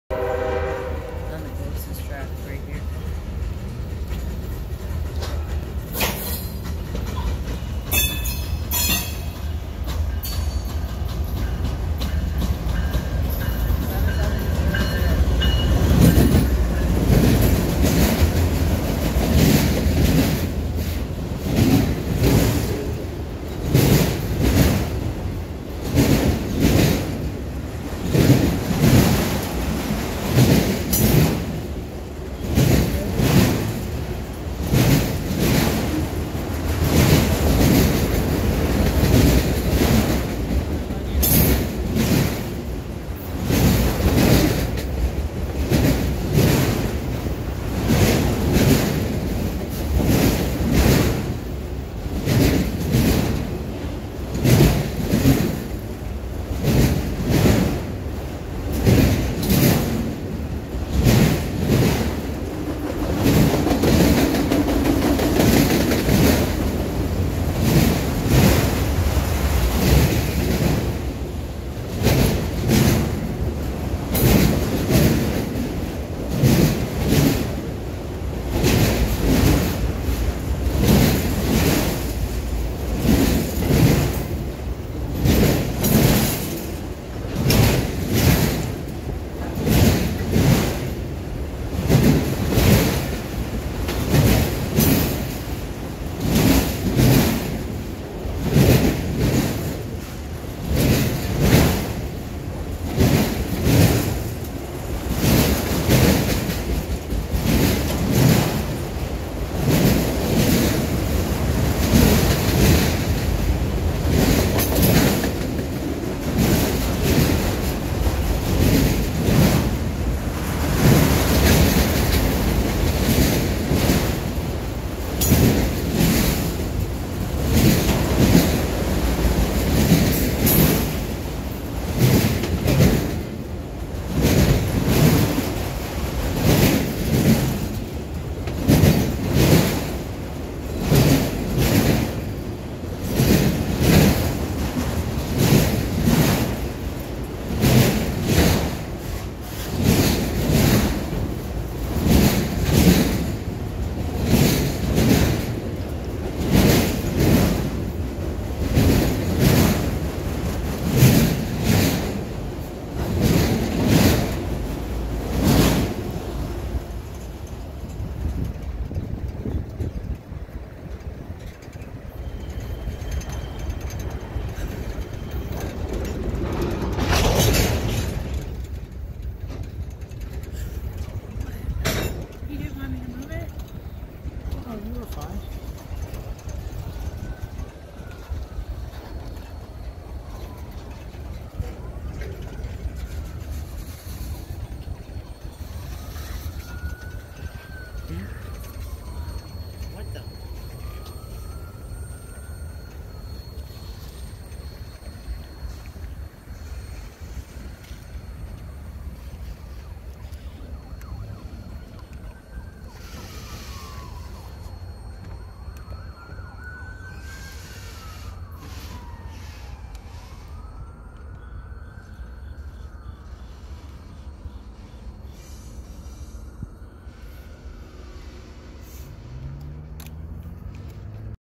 Wb race going into the yard
NS 7700 pulling long autorack train